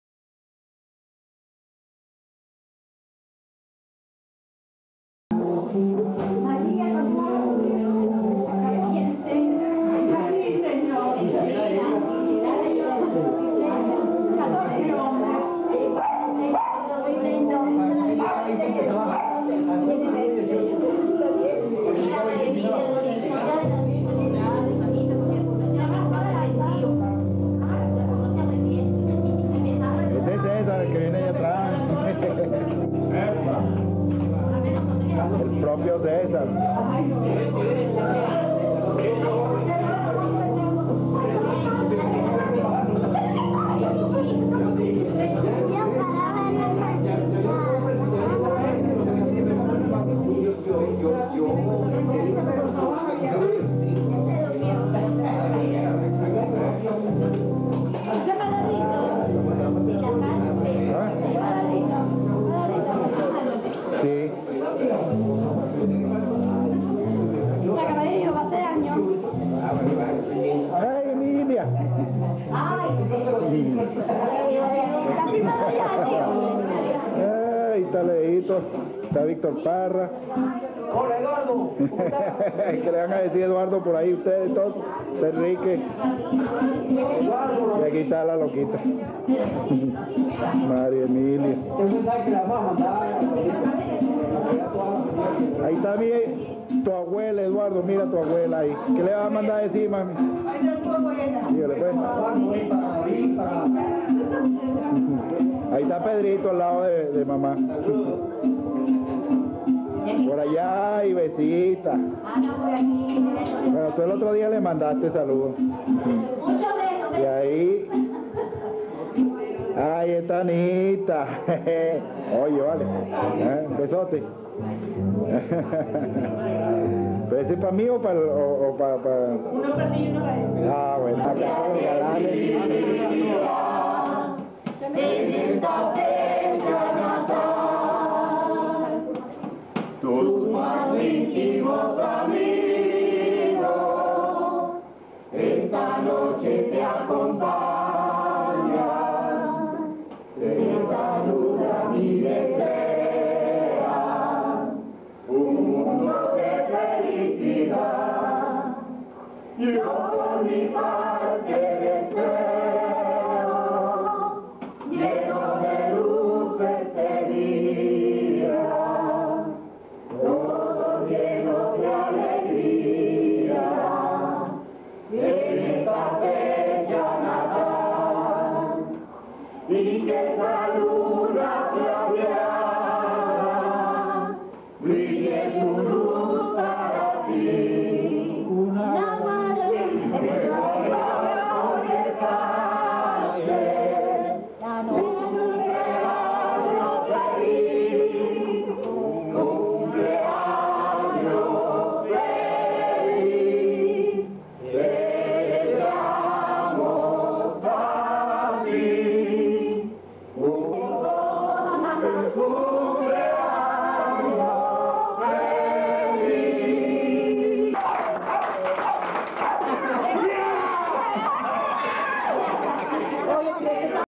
Cumpleaños de Tio Victor Tinoco 1992
En este video estan muchos familiares queridos, en especial nuestra linda abuela Lilia!!
Gracias a Alexander por enviarme este video hace años!